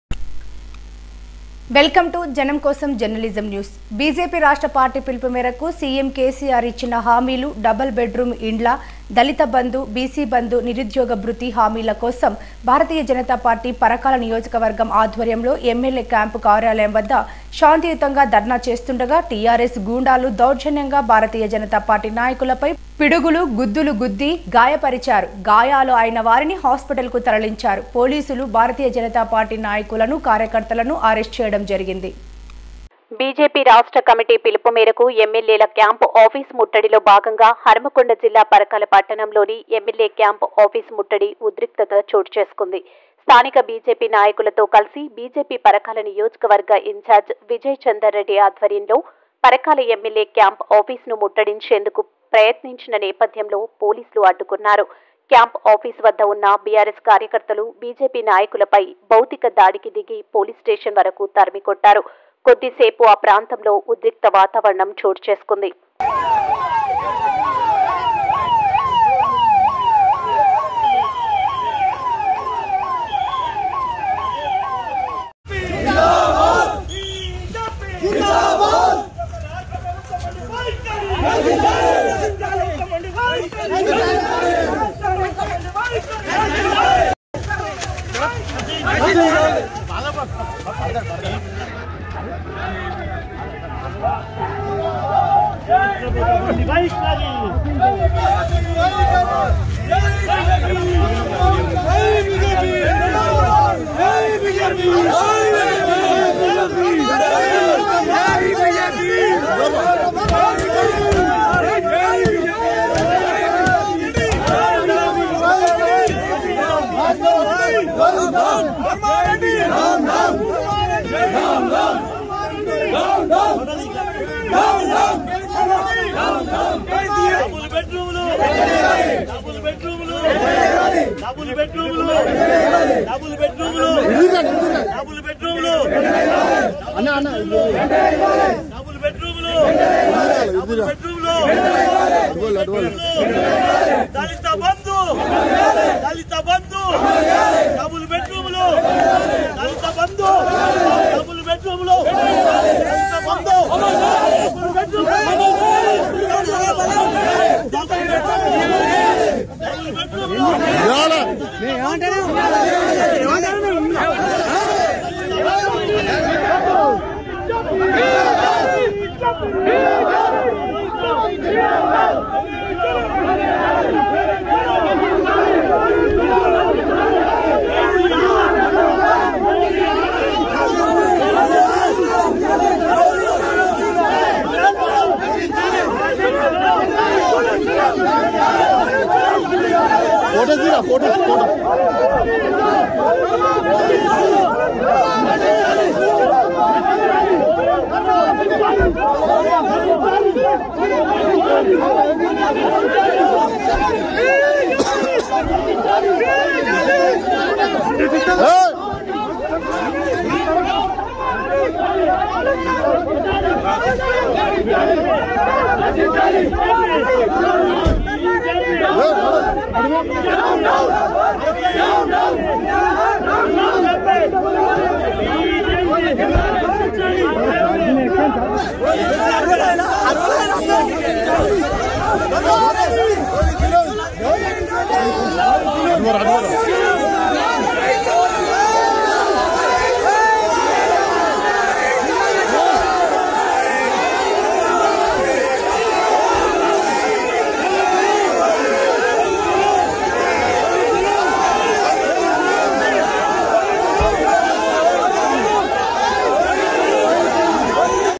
బిజెపి రాష్ట్ర పార్టీ పిలుపుమేరకు సీఎం కేసీఆర్ ఇచ్చిన హామీలు/డబుల్ బెడ్ రూమ్ ఇండ్ల దళిత బంధు, బీసీ..
*ఫ్లాష్ ఫ్లాష్ ఫ్లాష్ ఫ్లాష్*

*పరకాల ఎమ్మెల్యే క్యాంప్ కార్యాలయం ముట్టడి*

 *రాష్ట్ర కార్యవర్గ సభ్యులు డాక్టర్  పెసరు విజయచందర్ రెడ్డి గారు అరెస్ట్*

బిజెపి రాష్ట్ర పార్టీ పిలుపుమేరకు సీఎం కేసీఆర్ ఇచ్చిన హామీలు/డబుల్ బెడ్ రూమ్ ఇండ్ల , దళిత బంధు, బీసీ బందు, నిరుద్యోగ భృతి హామీల కోసం భారతీయ జనతా పార్టీ పరకాల నియోజకవర్గం ఆధ్వర్యంలో ఎమ్మెల్యే క్యాంపు కార్యాలయం వద్ద శాంతియుతంగా ధర్నా చేస్తుండగా టిఆర్ఎస్ గుండాలు దౌర్జన్యంగా భారతీయ జనతా పార్టీ నాయకులపై పిడుగులు గుద్దులు గుద్ది గాయపరిచారు.
గాయాలు అయిన వారిని హాస్పిటల్ కు తరలించారు
పోలీసులు భారతీయ జనతా పార్టీ  నాయకులను, కార్యకర్తలను అరెస్టు చేయడం జరిగింది